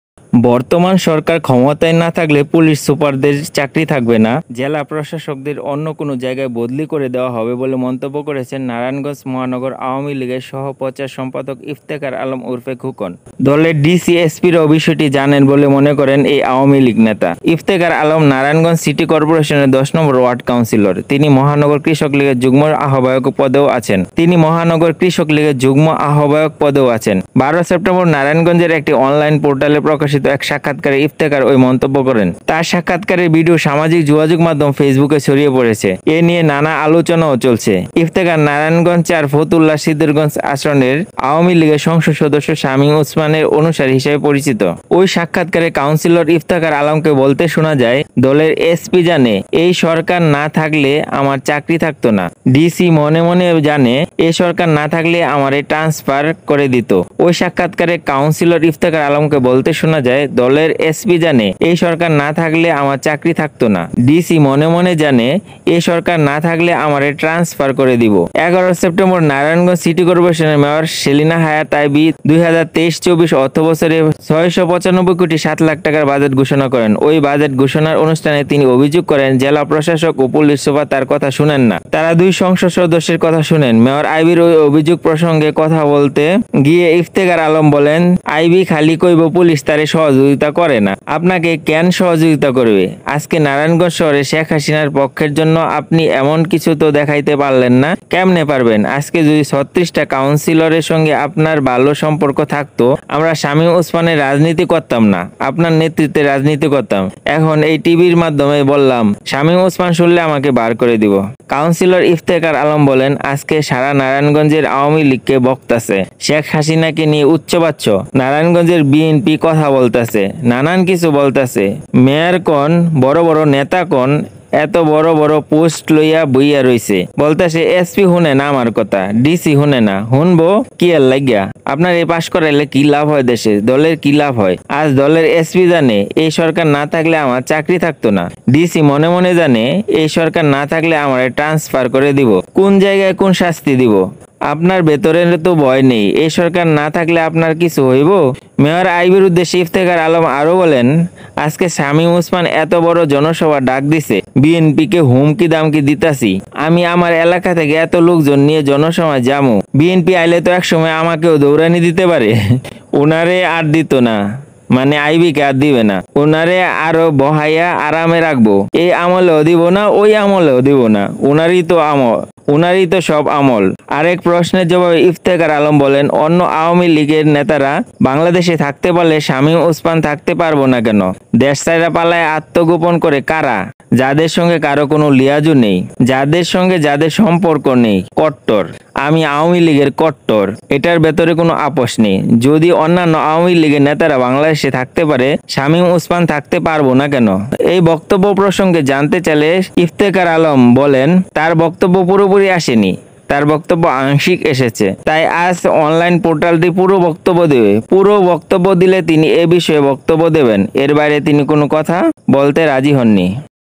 'এই সরকার না থাকলে এসপিদের চাকরি থাকবে না, ডিসিদের বদলি হবে'। শামীম ওসমান। আইভী রহমান।
'এই সরকার না থাকলে এসপিদের চাকরি থাকবে না, ডিসিদের বদলি হবে'-আওয়ামী লীগ নেতার বক্তব্য ভাইরাল

নারায়ণগঞ্জের মেয়র সেলিনা হায়াৎ আইভী বলেন, ডিসি, এসপি আমার কথা শুনে না।